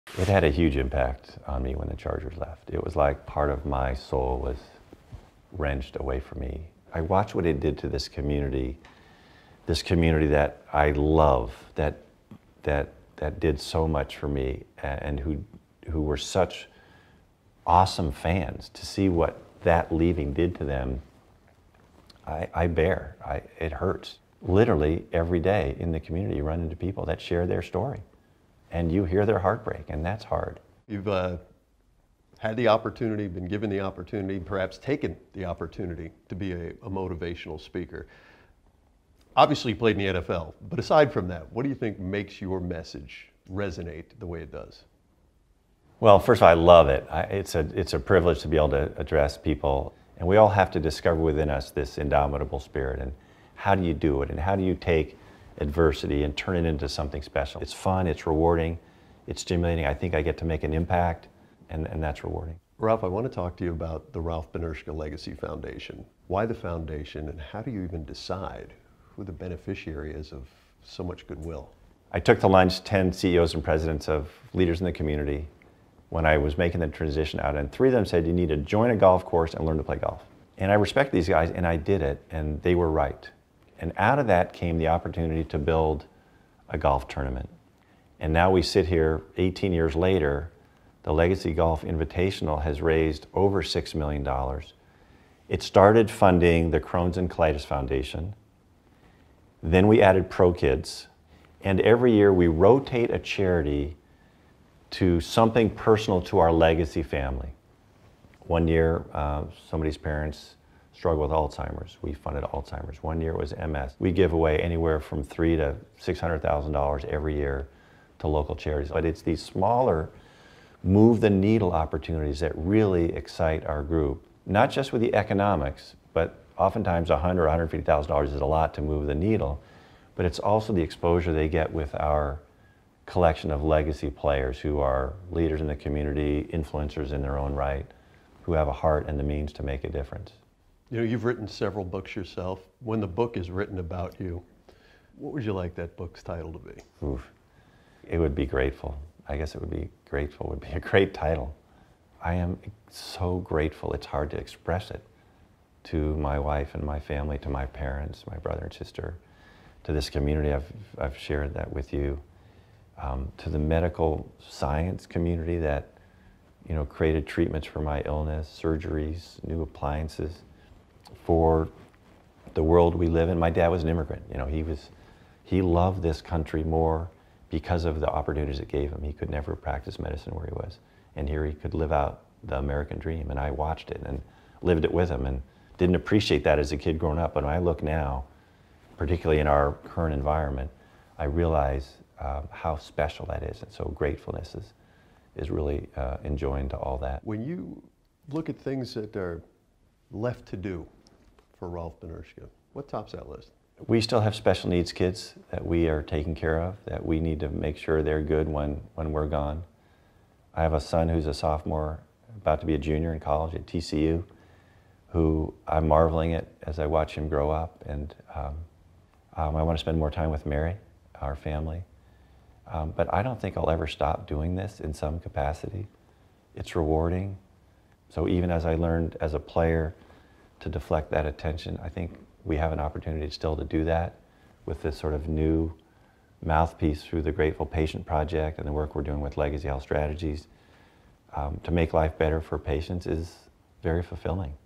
Chargers' HOFer saddened when team left for LA
Rolf Benirschke talked about being heartbroken when the Chargers left San Diego.